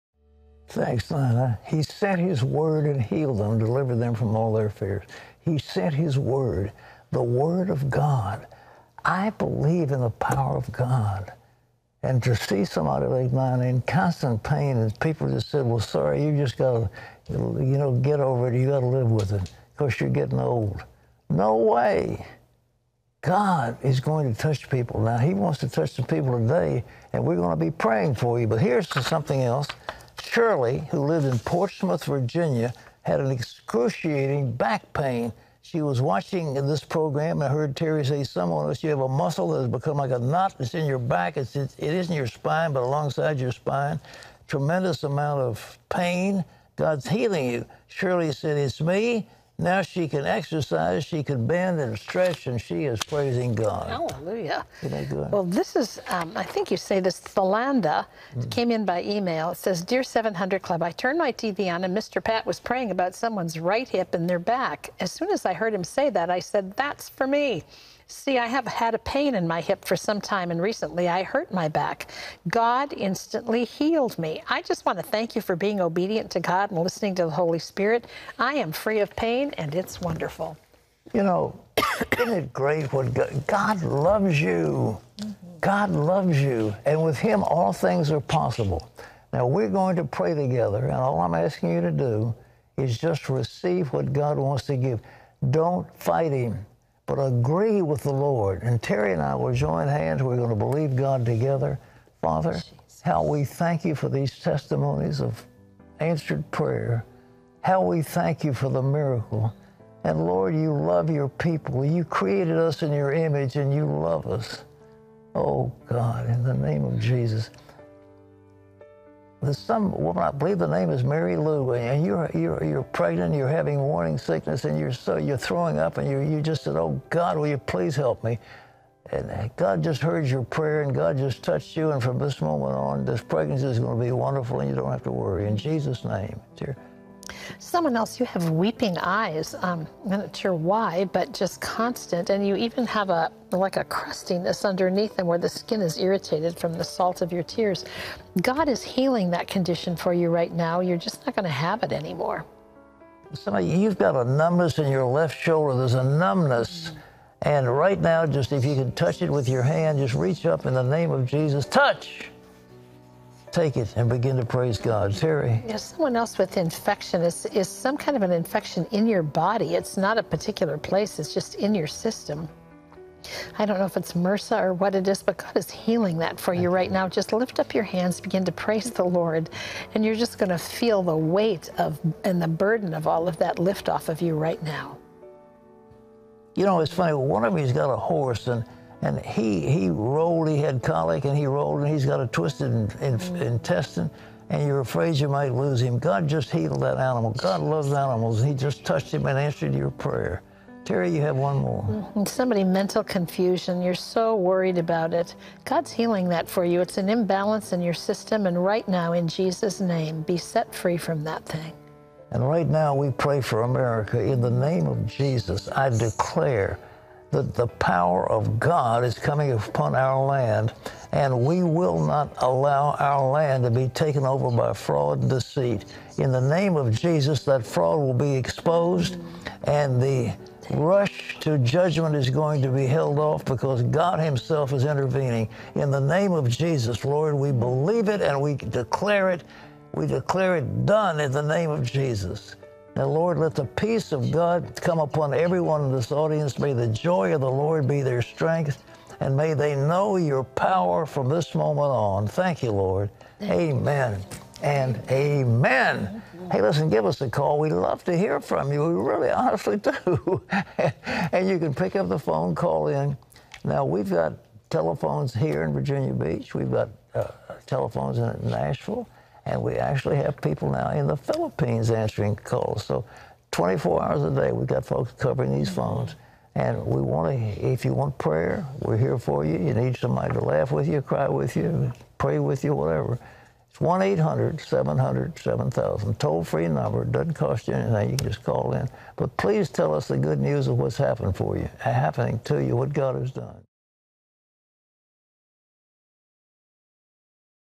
Praying For Your Needs: December 7, 2020
Pat and Terry pray for you and deliver words of knowledge through the power of the Holy Spirit for your healing.

► CLICK to experience God’s love, purpose and forgiveness in your life